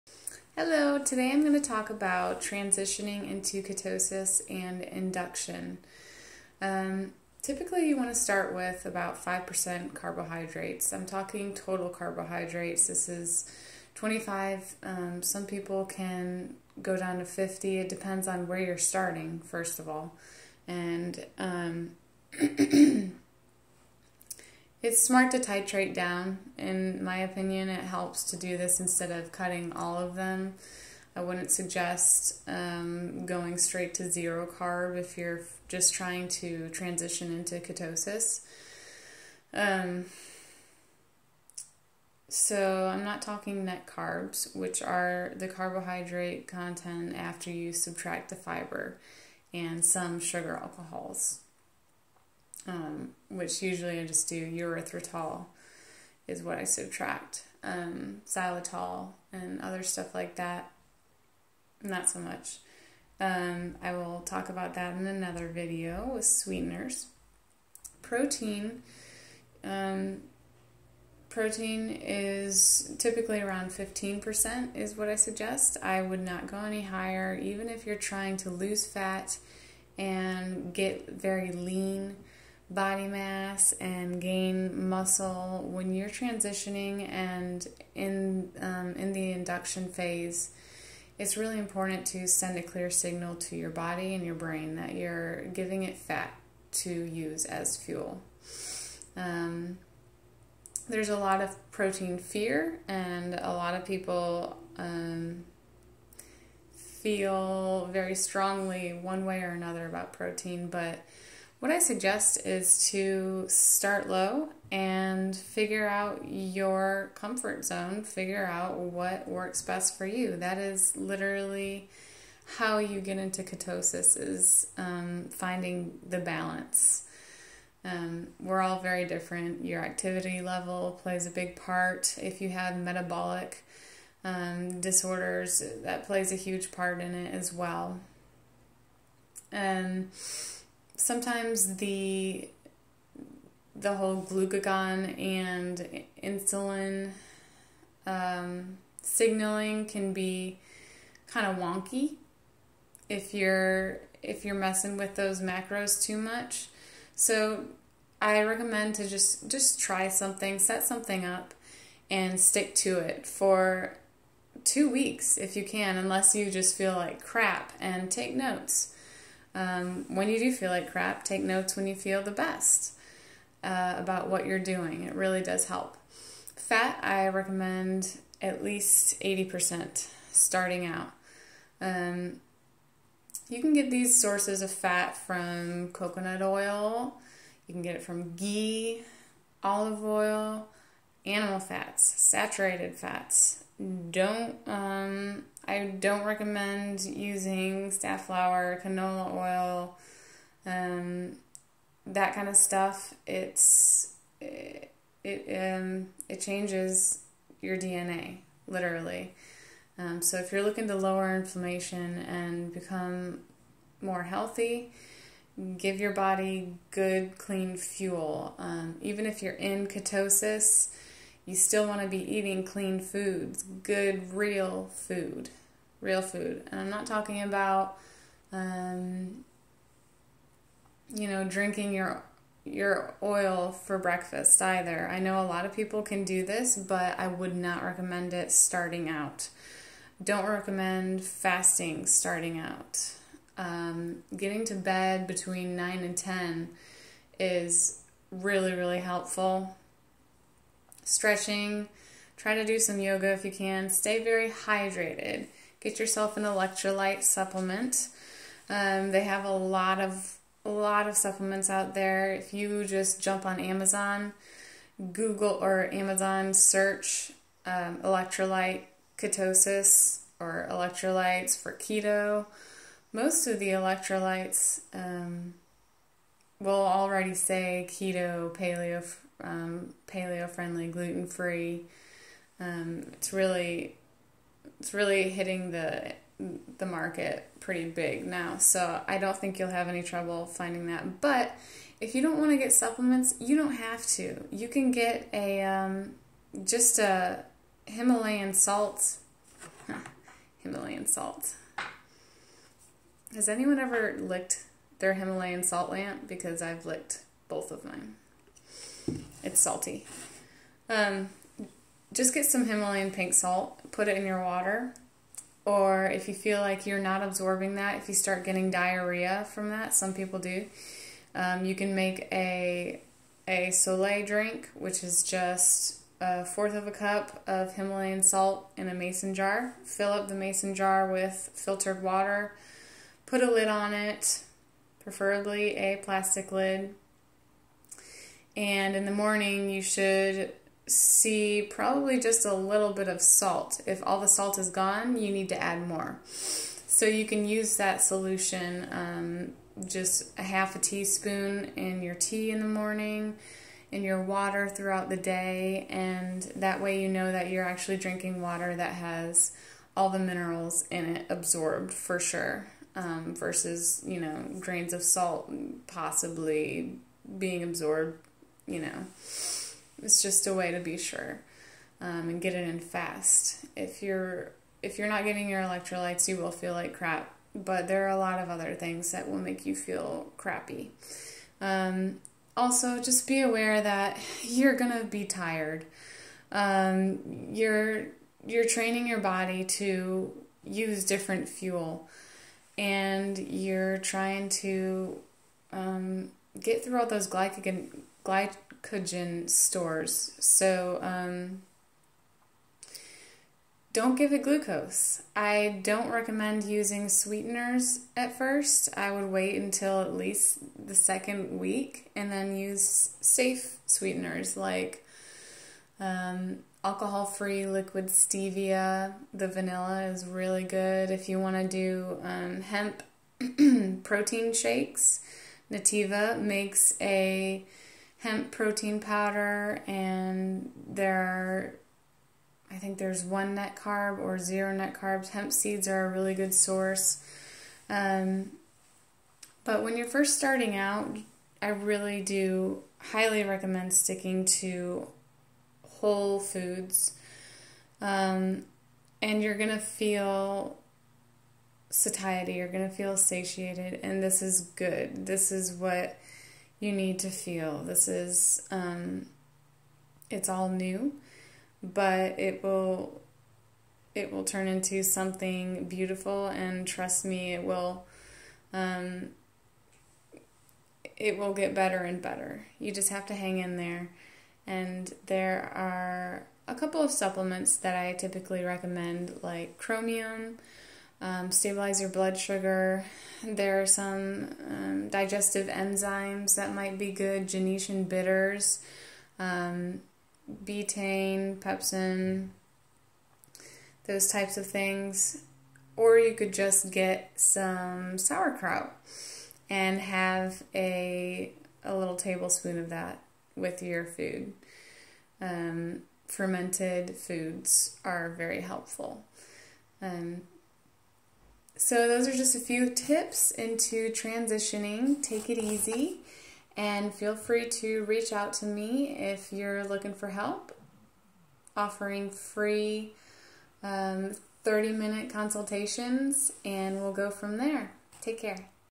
The Transition/ Keto Induction
The gist of getting into ketosis for newbies, and tips for an easy transition. Keep calm and keto on!

I hope to inspire others who are fighting systemic and chronic or other health related goals, and to encourage wellness for all.

Be well.